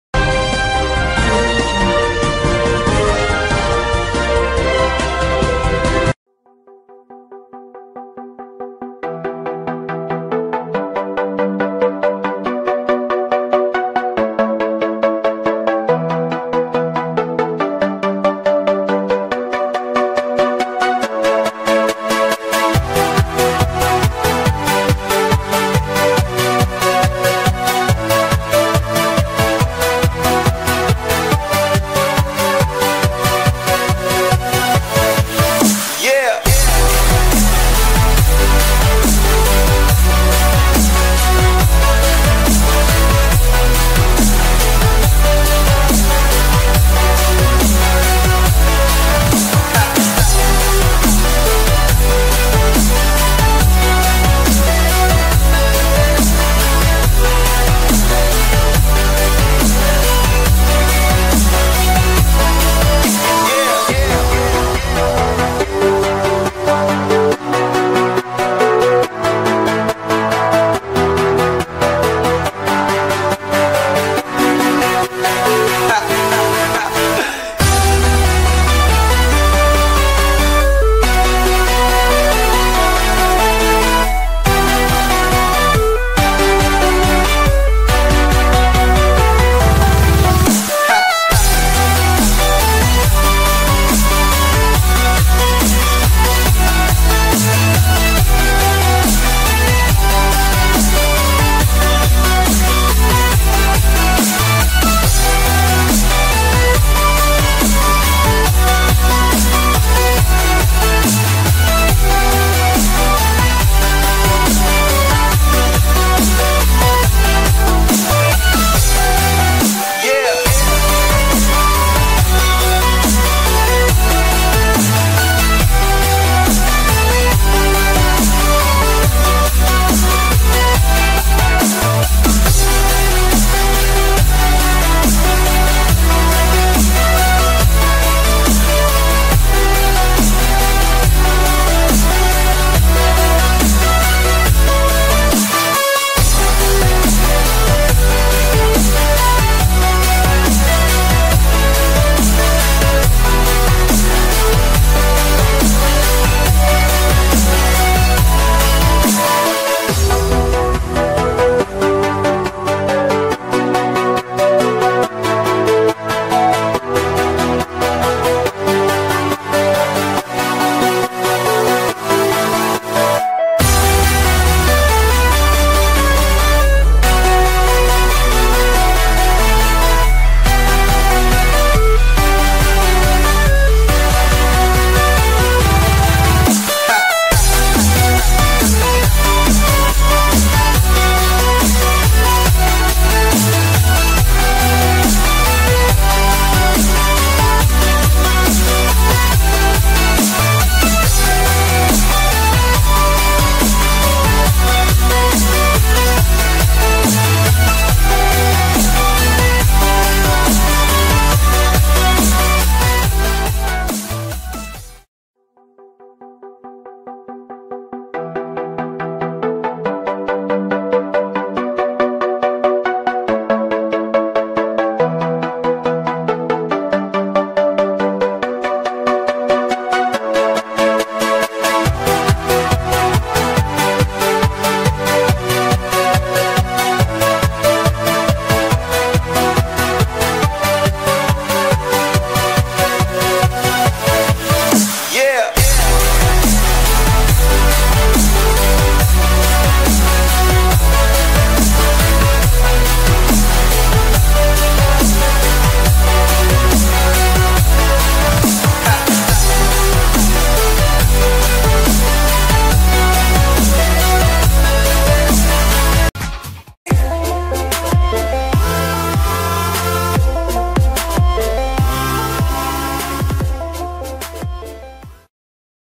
Barcelona news: Lionel Messi hands Arsenal and Liverpool boost by ‘wanting Dembele sold'
► Barcelona star Dembele has emerged as a target for Arsenal and Liverpool ahead of the summer transfe...
► PLL:
► Photo & Content Source :